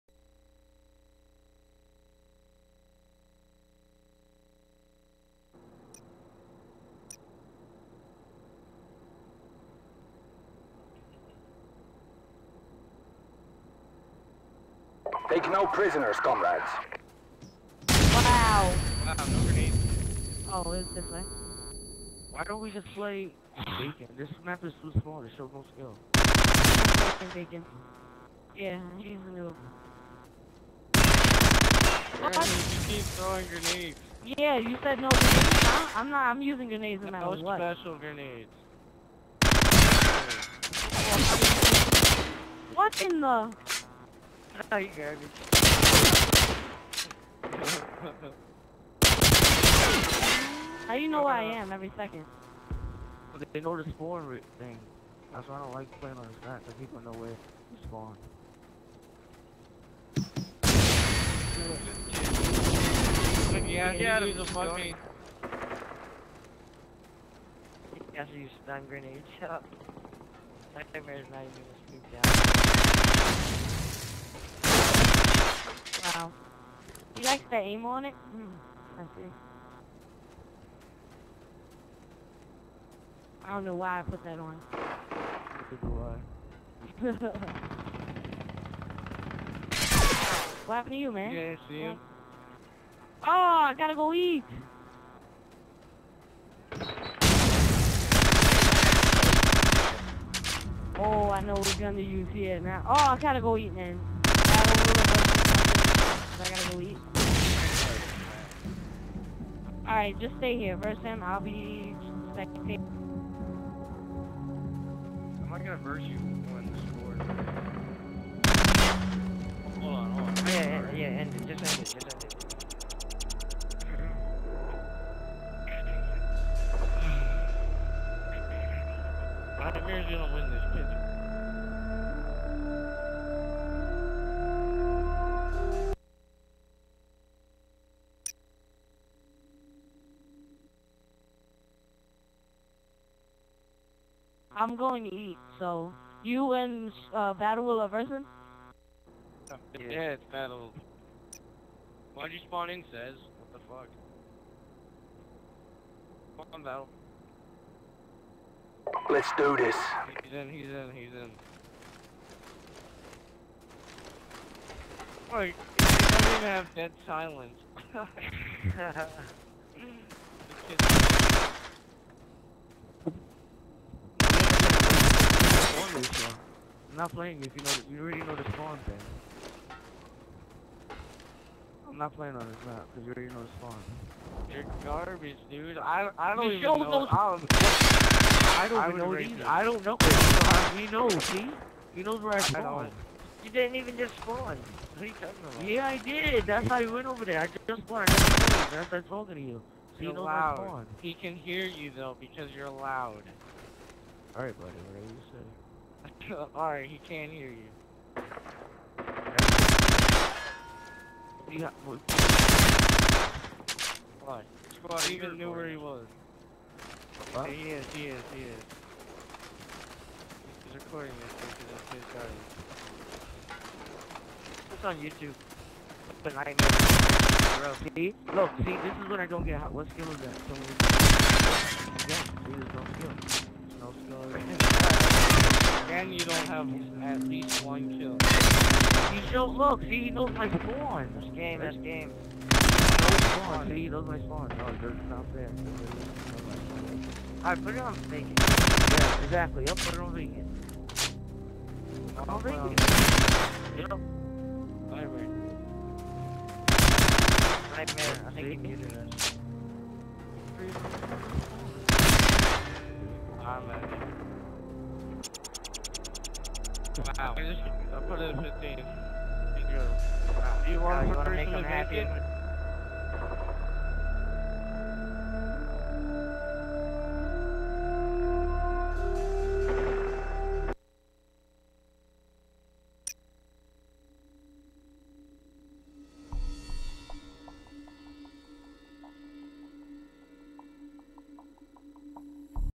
Vt gets raped by PO
These kids talk trash about us in a public lobby, then we bring them in a private 1v1.